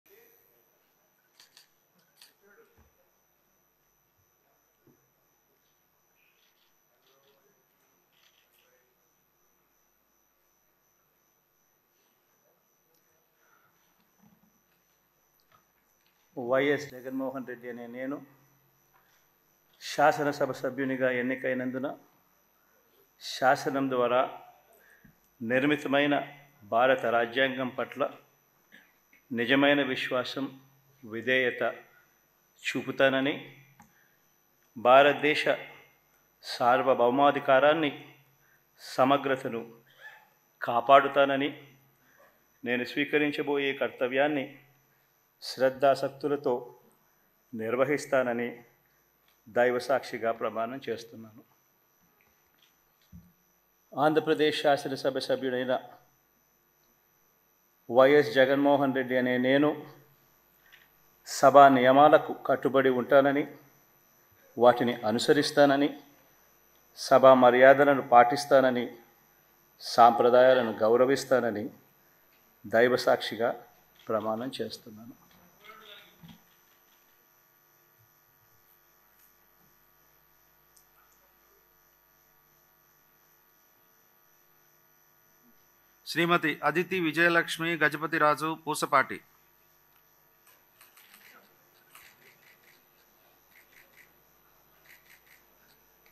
ఎమ్మెల్యేలుగా ప్రమాణస్వీకారం చేసిన జగన్ | YS Jagan Takes Oath as MLA in Assembly | Aadhan
Watch ఎమ్మెల్యేలుగా ప్రమాణస్వీకారం చేసిన జగన్ | YS Jagan Takes Oath as MLA in Assembly | Aadhan